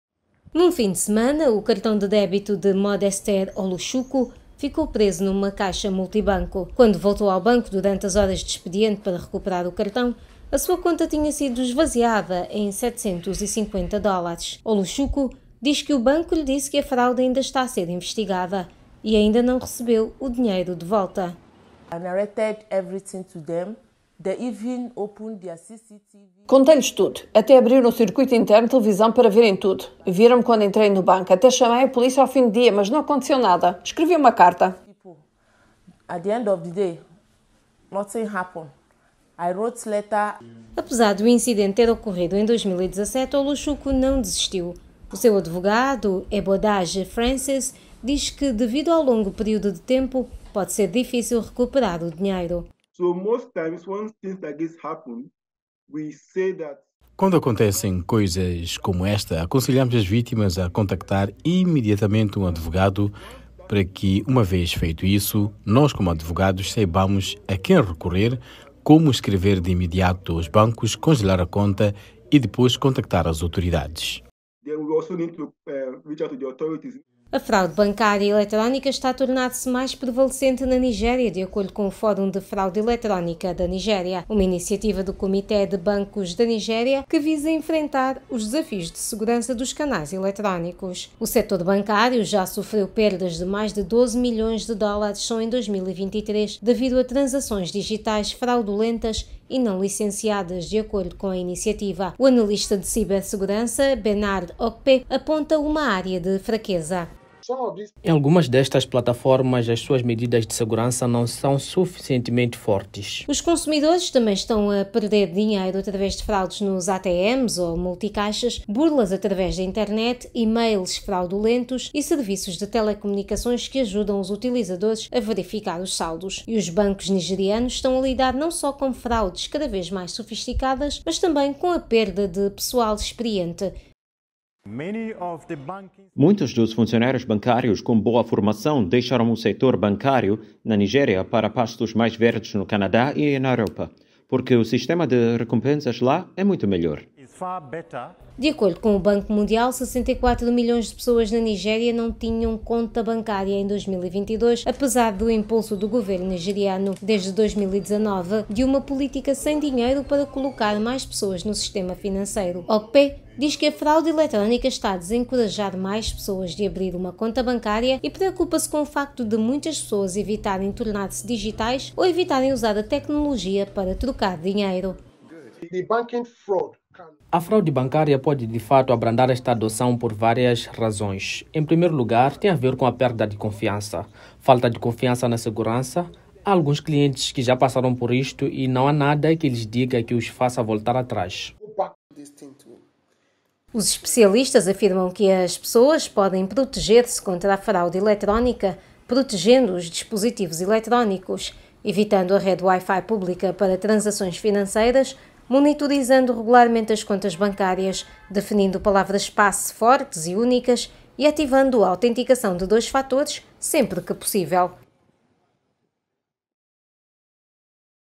Bancos da Nigéria acrescentam a fraude eletrónica à lista de problemas
De acordo com o Banco Mundial, em 2022, 64 milhões de pessoas na Nigéria não tinham uma conta num banco ou noutra instituição financeira respeitável. Mas as que têm estão a ser vítimas de fraude bancária eletrónica, ou e-fraude